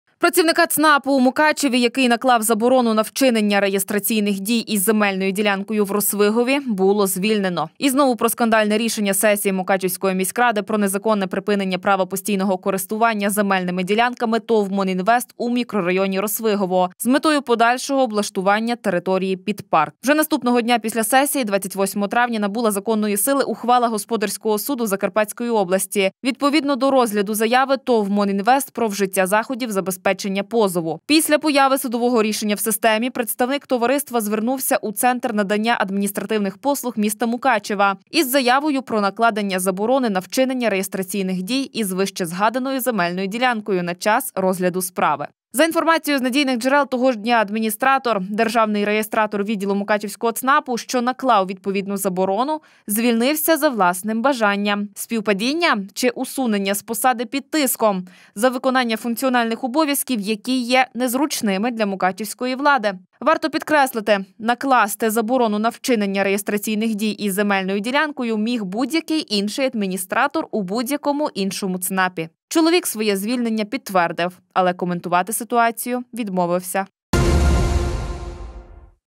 Звільнення у ЦНАПі. Добровільно-примусово?
Працівника ЦНАПу у Мукачеві, який наклав заборону на вчинення реєстраційних дій із земельною ділянкою у Росвигові було звільнено. І знову про скандальне рішення сесії Мукачівської міськради про незаконне припинення права постійного користування земельними ділянками ТОВ «Мон-Інвест» у мікрорайоні «Росвигово», з метою подальшого облаштування території під парк.Вже наступного дня після сесії, 28 травня,  набула законної сили Ухвала Господарського суду Закарпатської області, відповідно до розгляду заяви ТОВ «Мон-Інвест» про вжиття заходів забезпечення позову. Після появи судового рішення в системі, представник товариства звернувся у Центр надання адмінпослуг міста Мукачева із заявою про накладення заборони на вчинення реєстраційних дій із вищезгаданою земельною ділянкою на час розгляду справи. За інформацією з надійних джерел, того ж дня адміністратор, державний реєстратор відділу Мукачівського ЦНАПу, що наклав відповідну заборону, звільнився за власним бажанням. Співпадіння, чи усунення з посади під тиском, за виконання функціональних обов’язків, які є незручними для мукачівської влади? Варто підкреслити, накласти заборону на вчинення реєстраційних дій із земельної ділянкою міг будь-який інший адміністратор у будь-якому іншому ЦНАПі. Чоловік своє звільнення підтвердив, але коментувати ситуацію відмовився.